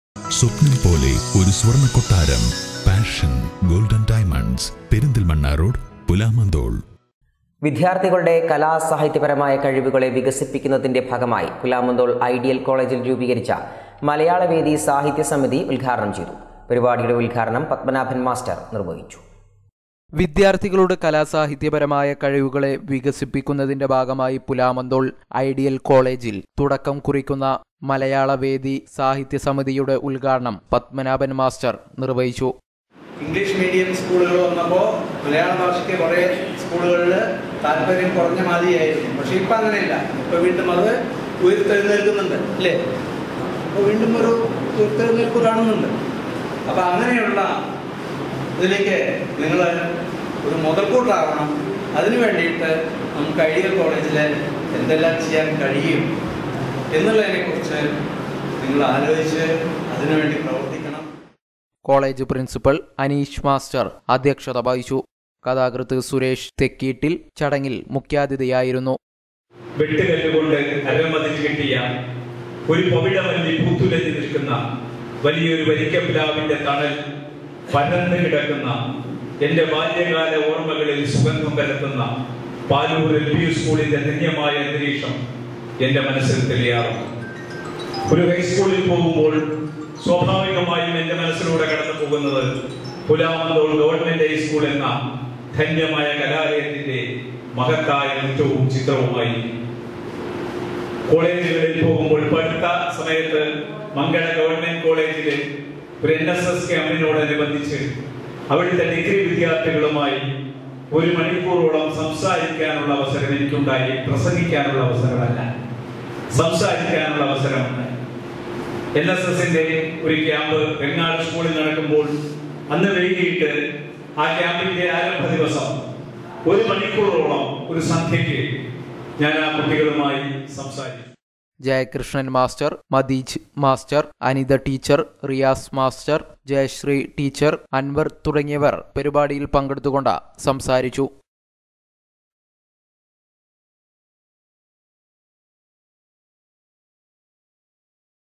പുലാമന്തോൾ ഐഡിയൽ കോളേജിൽ രൂപീകരിച്ച 'മലയാള വേദി' സാഹിത്യ സമിതിയുടെ ഉദ്ഘാടന ചടങ്ങ് സംഘടിപ്പിച്ചു
വിദ്യാർത്ഥികളുടെ കലാസാഹിത്യപരമായ കഴിവുകളെ വികസിപ്പിക്കുന്നതിന്റെ ഭാഗമായി പുലാമന്തോൾ ഐഡിയൽ കോളേജിൽ രൂപീകരിച്ച 'മലയാള വേദി' സാഹിത്യ സമിതിയുടെ ഉദ്ഘാടന ചടങ്ങ് സംഘടിപ്പിച്ചു.